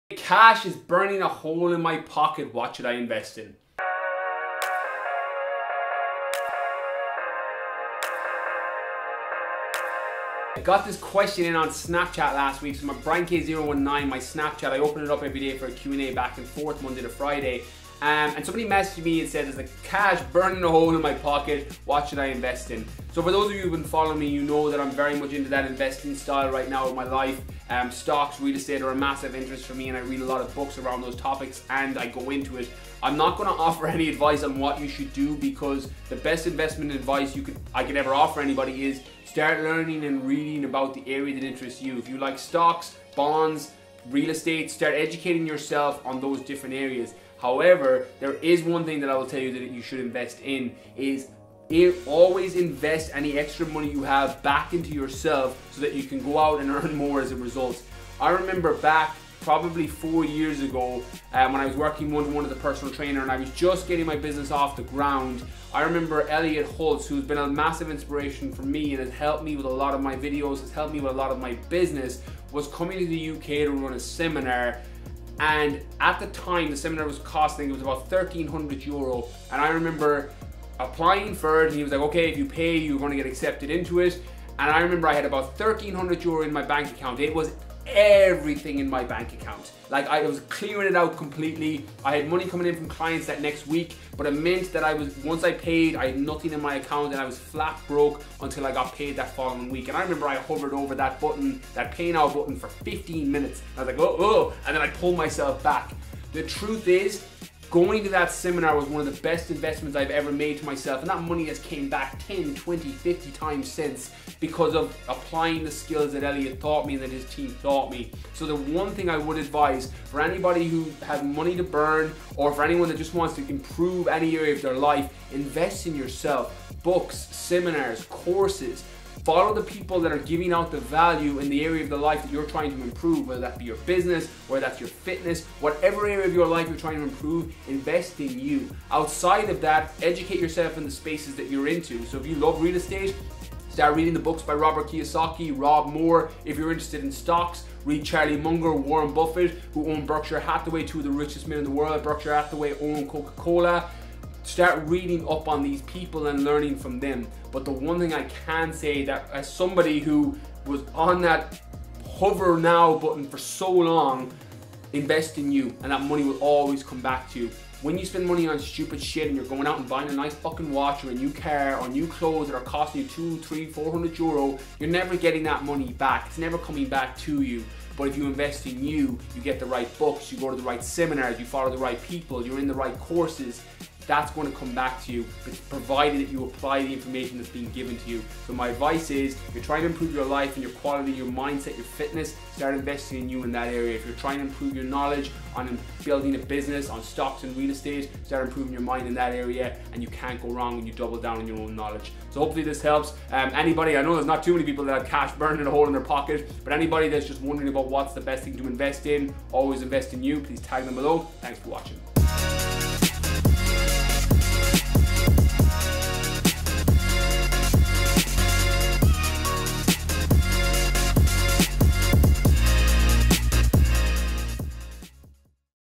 CASH IS BURNING A HOLE IN MY POCKET, WHAT SHOULD I INVEST IN?
Personally, I’m a massive fan of stocks and real estate and although it's rare that someone has a hole burning in their pocket but regardless of whether you have £5 or £5million, this is what I would always invest my money in!